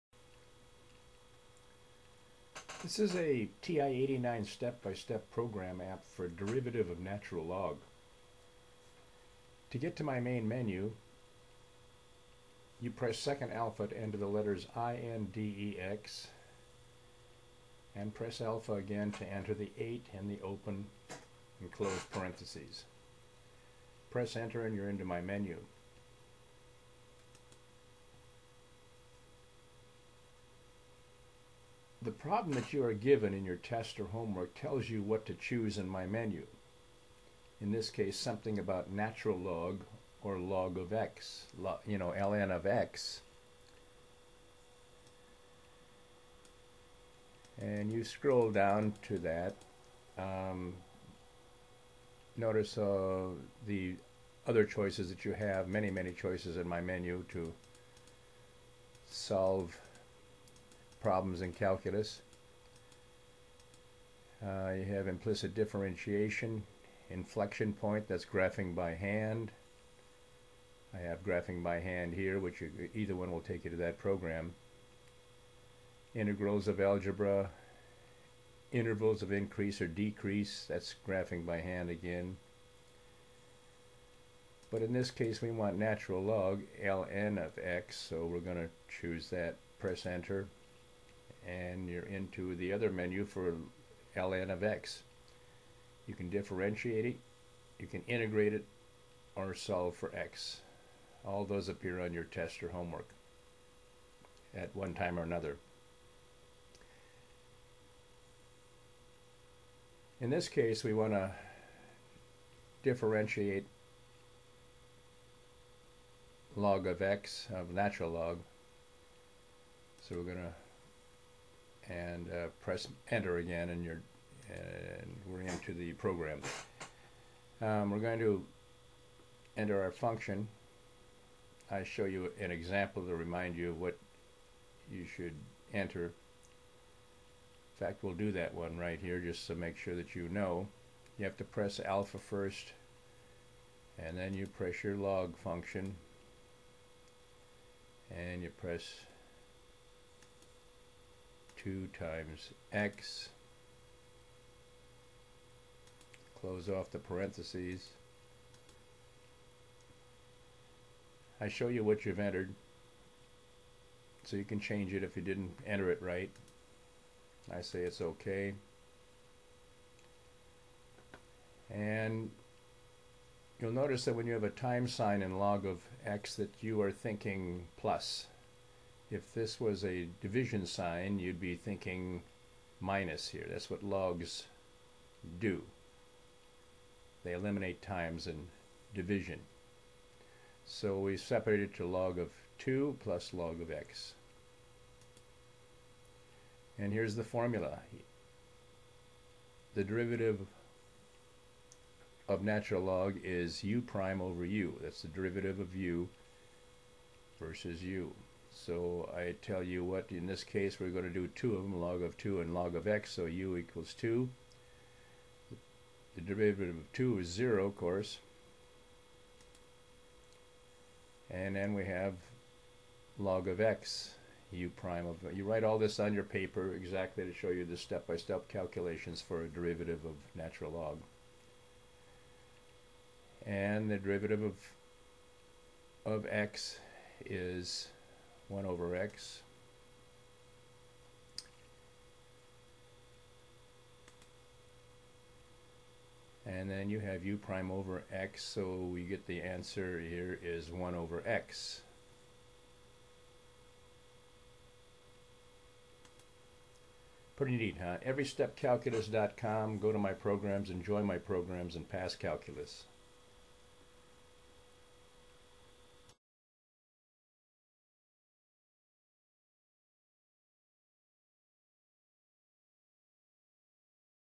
Derivative of Natural Log | Program App for TI 89 Titanium
A demonstration of solving calculus problems, in this case finding the derivative of a natural log problem, a calculus program app for the ti 89 titanium calculator, voyage 200 calculator, ti 92 plus calculator or Calculator showing step by step calculations for many calculus problems that occur in calculus tests and homework